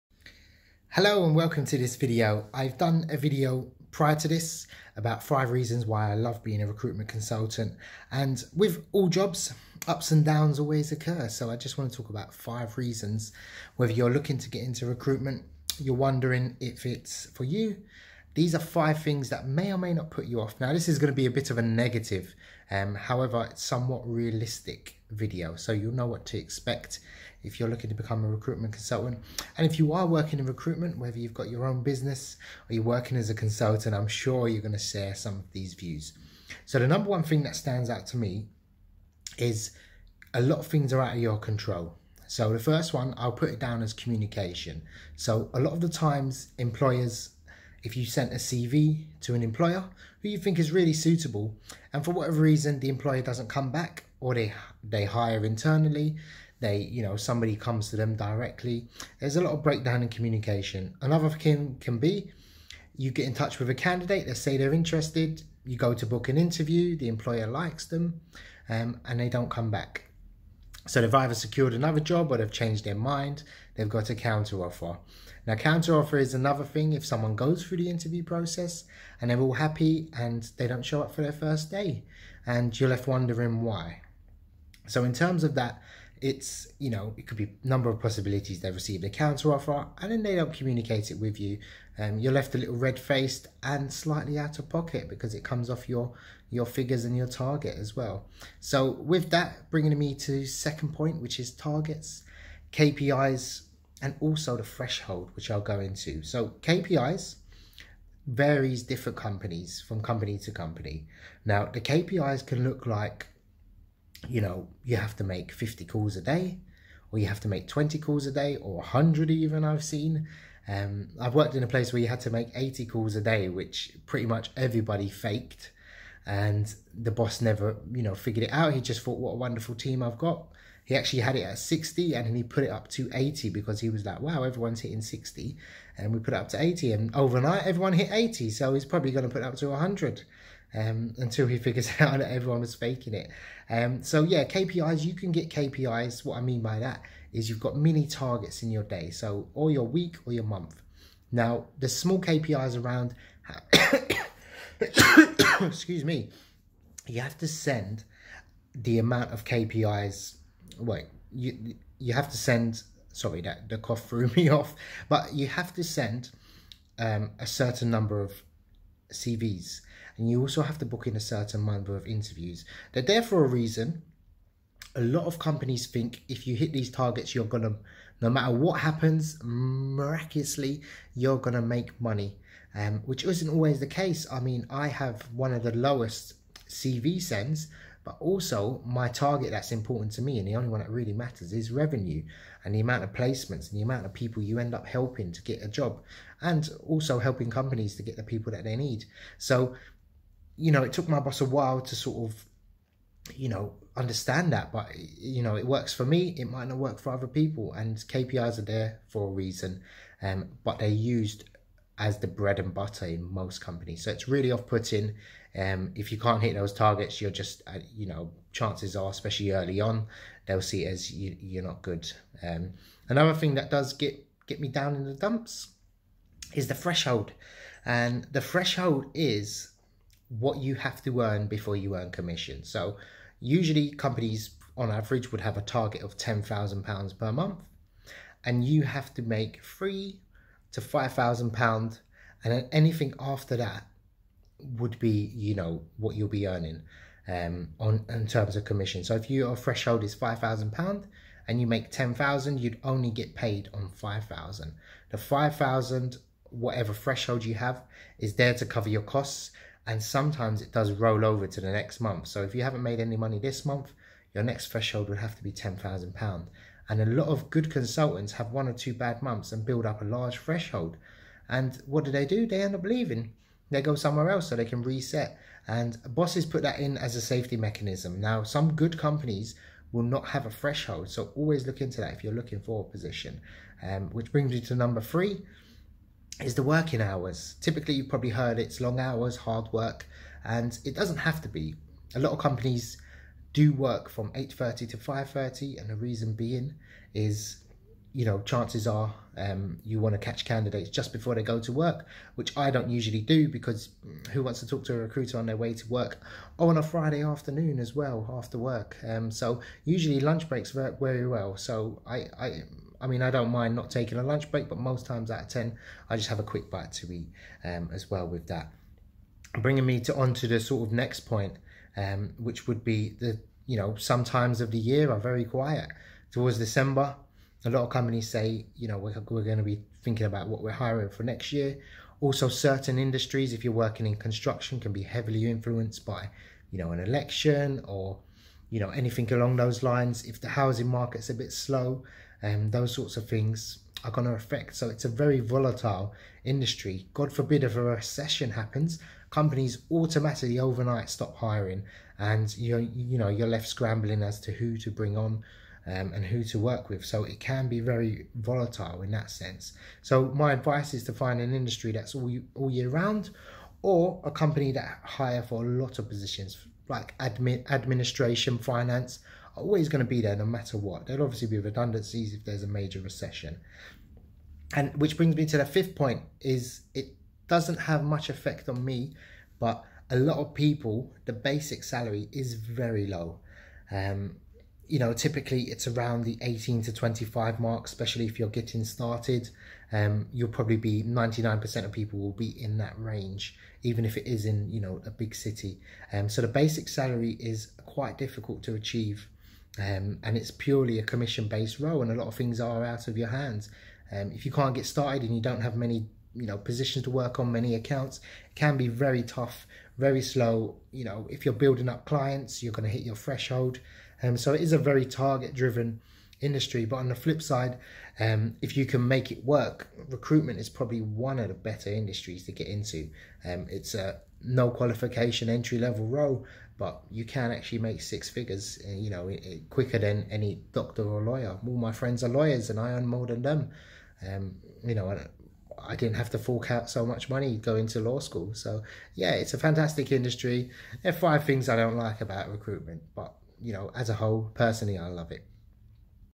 5 reasons why I hate working as a recruitment consultant
I made a video about 5 things I love about recruitment. So it only makes sense to explain the negatives about working in the recruitment industry.